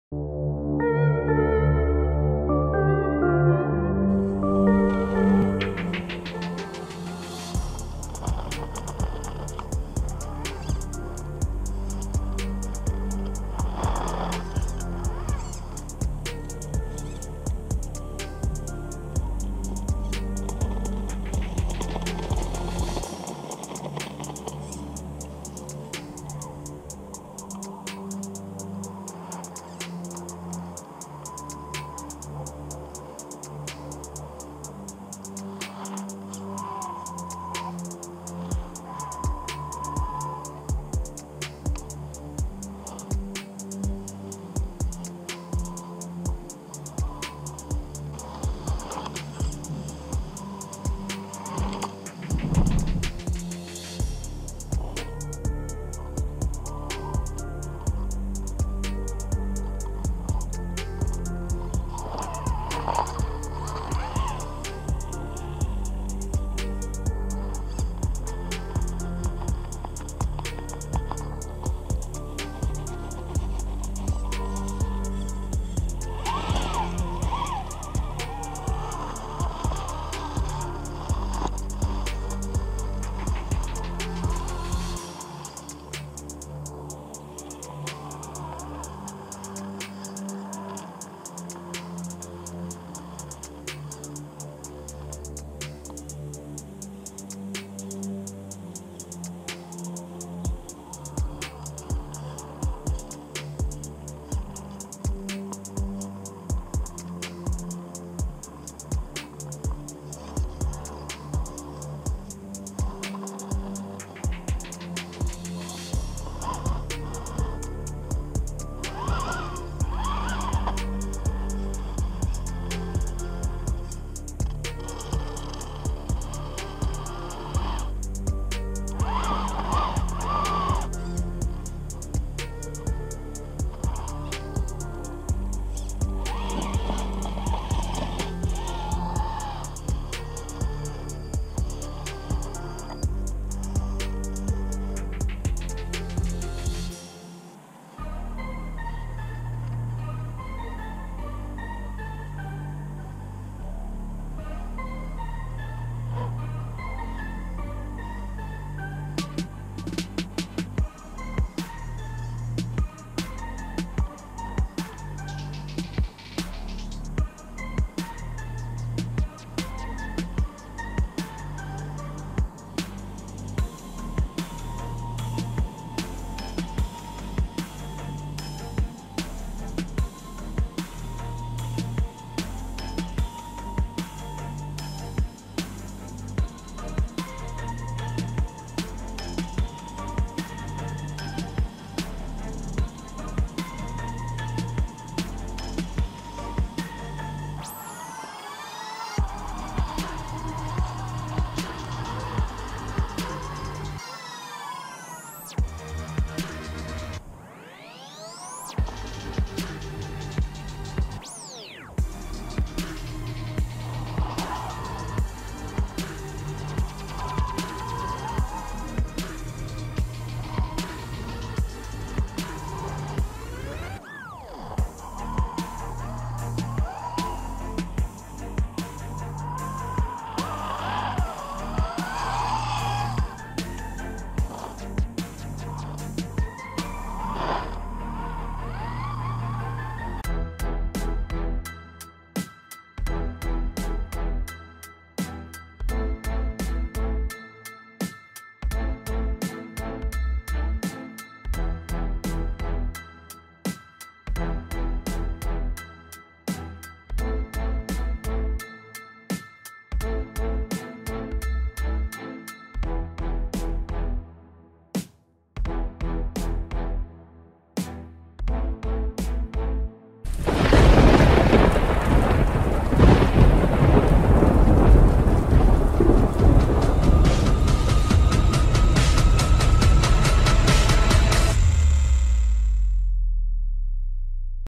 TRX4 sport, rock crawling, holmes 55T,Jeep hard body, Drôme, France
No slow motion!
TRX4 Sport 315 mm changed to 324 mm,
Hard Body jeep 315 mm, Tyres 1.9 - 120mm
Motor Holmes 55T, quickrun 1080,
No oil in shock,
Lipo 2s, 4000mAH,
Warn 8274 Winch,
Proline Tyres BFGoodrich Krawler T/A KX 1.9 G8